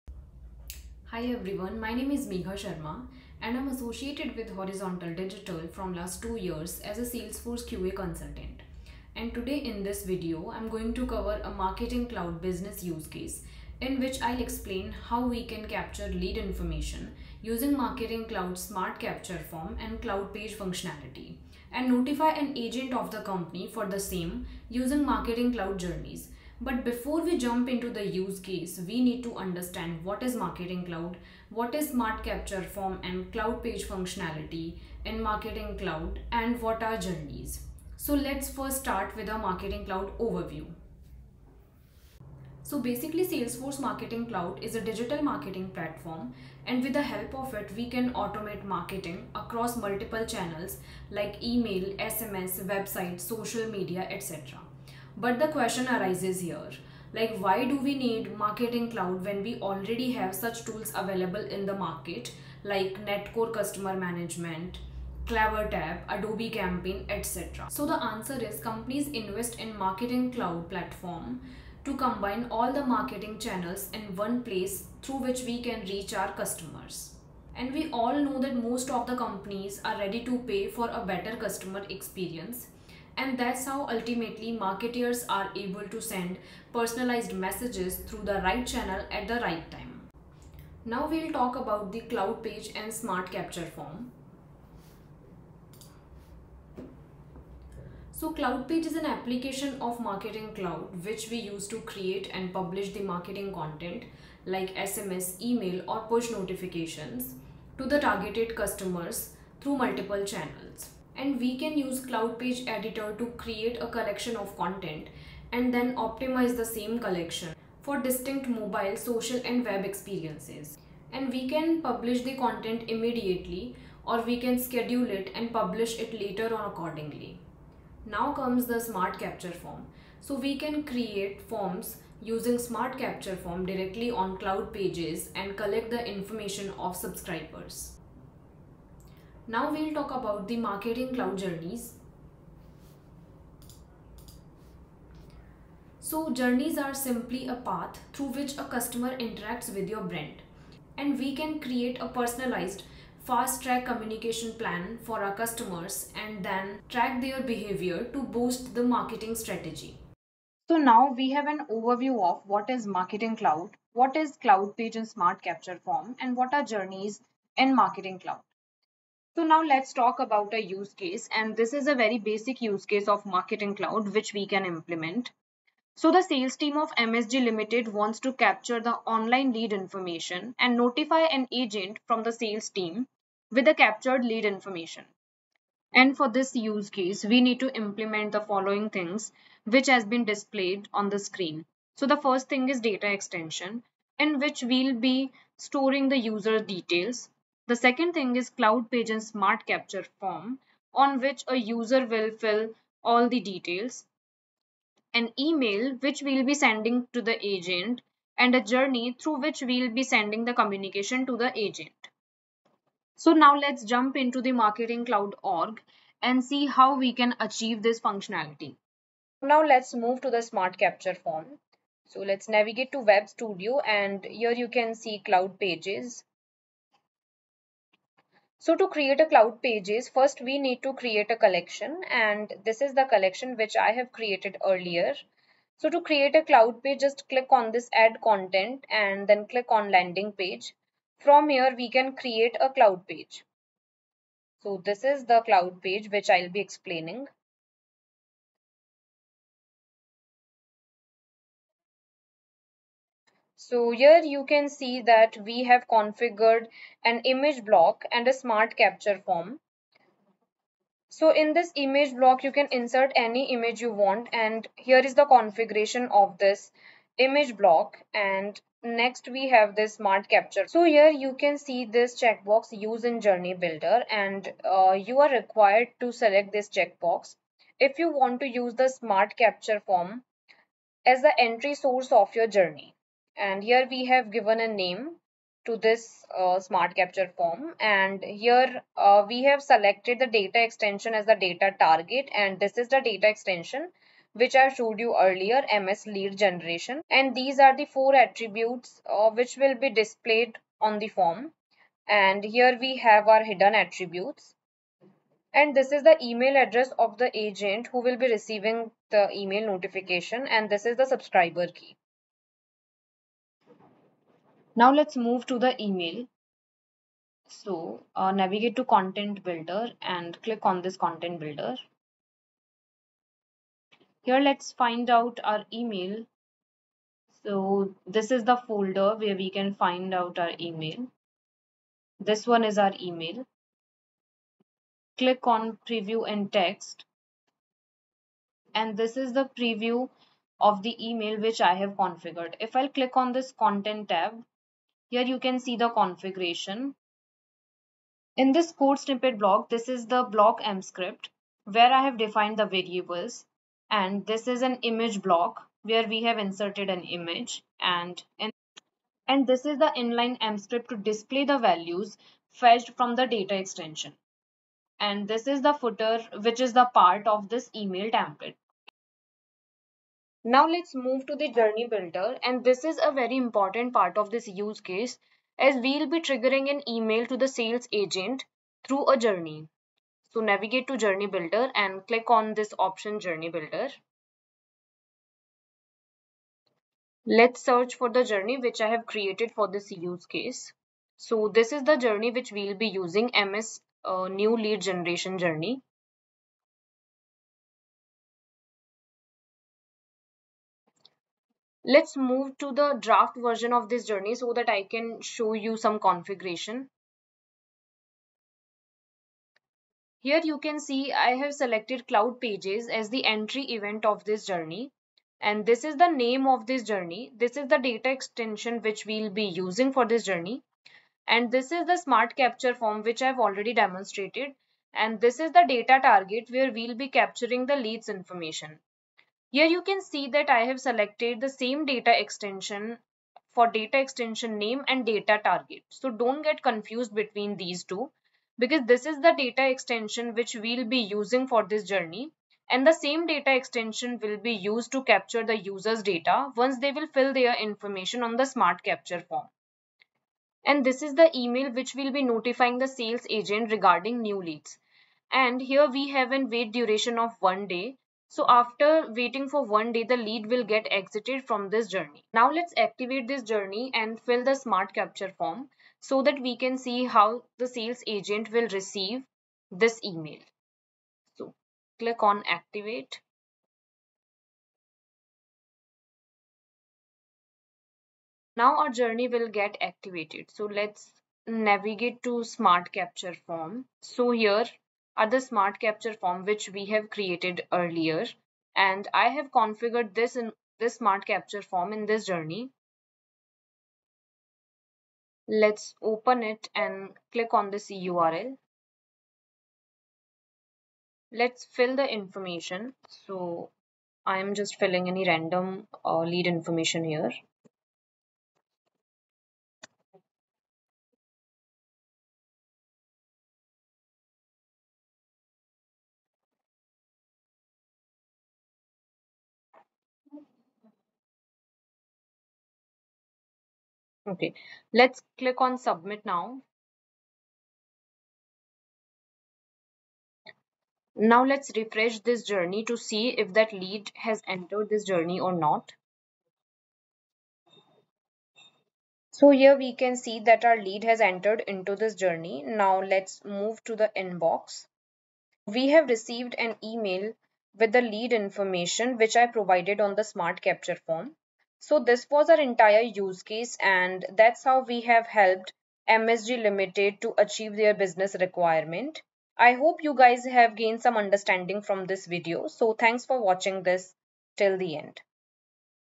Capture leads and notify through smart capture form, cloud pages, and journey.
In this use case, learn how to capture lead information using marketing cloud, smart capture form, and cloud page functionality and notify the agent using marketing cloud journeys.

0:26